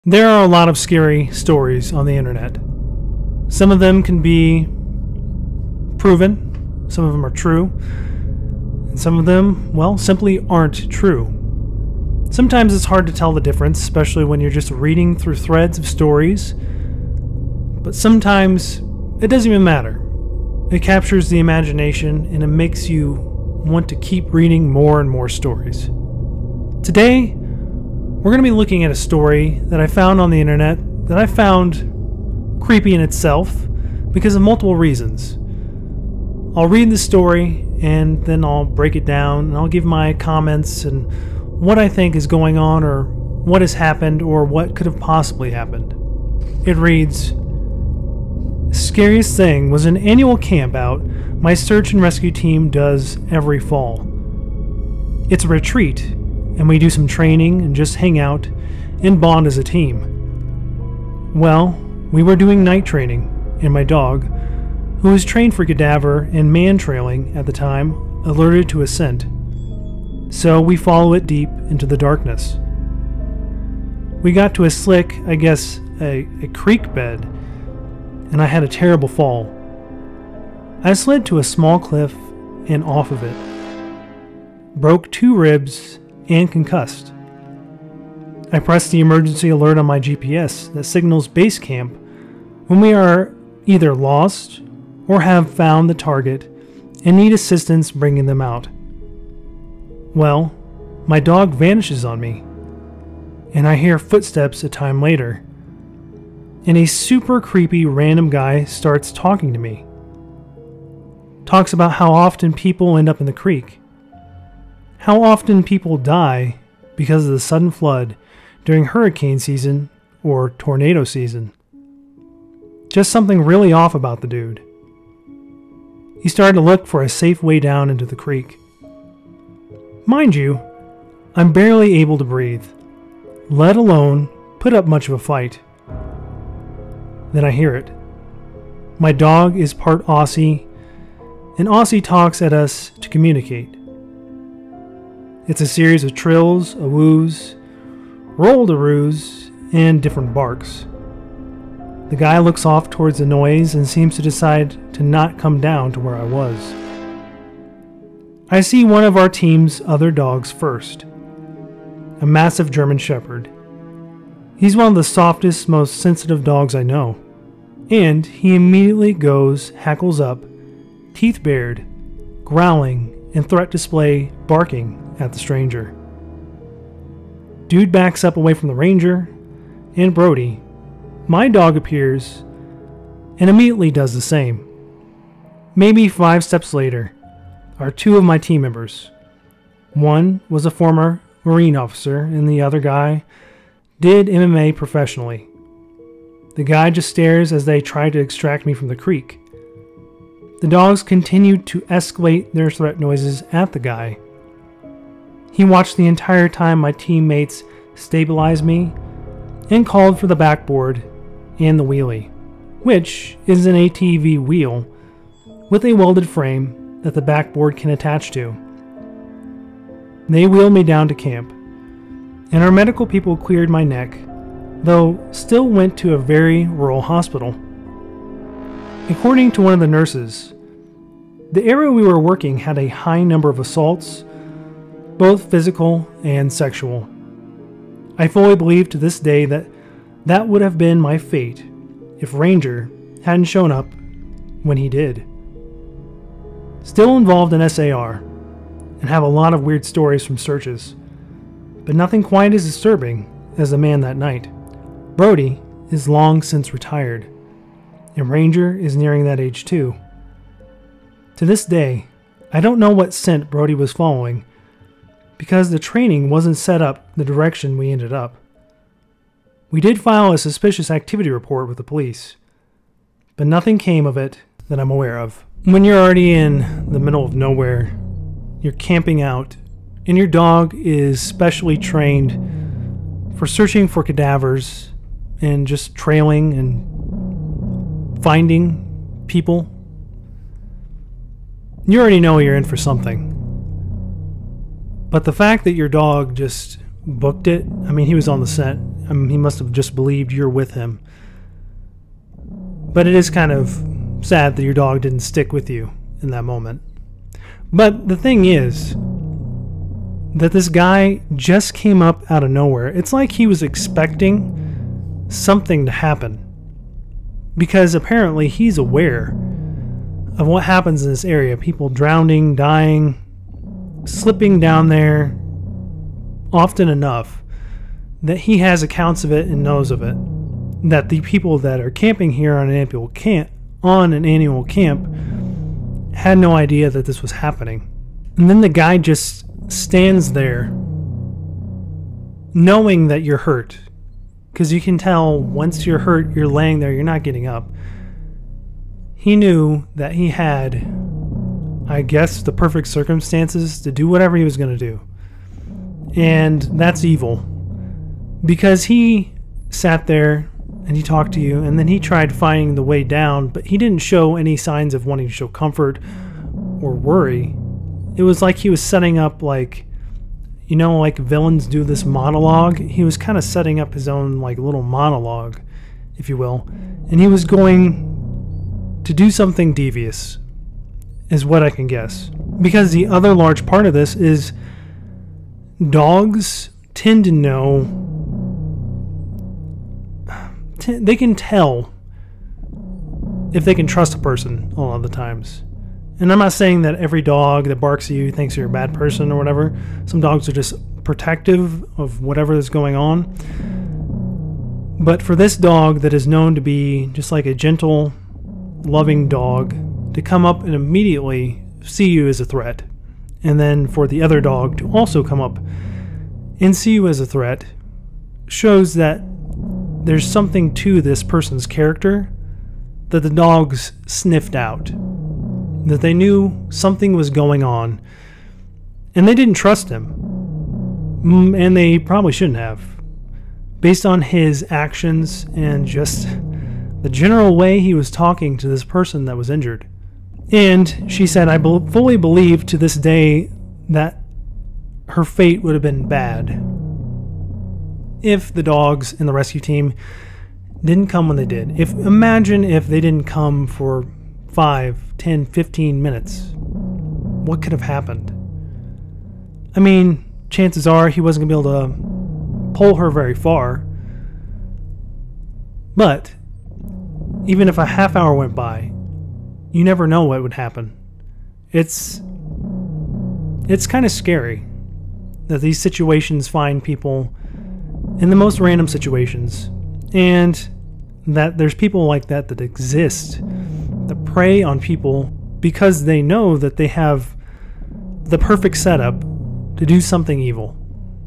When You’re Alone and Injured in the Woods…
When this person was on a annual camping trip with her dog and other colleagues they became injured and alone in the woods. Without warning a stranger came out of nowhere and further complicates the situation. This account lets you know how quickly a situation can become dangerous and scary when you're alone in the woods.

Chapters

00:00 Intro
00:46 Alone and Injured in the Woods
04:35 Analyzing the story
07:20 The Situation Could Have Gone South/Outro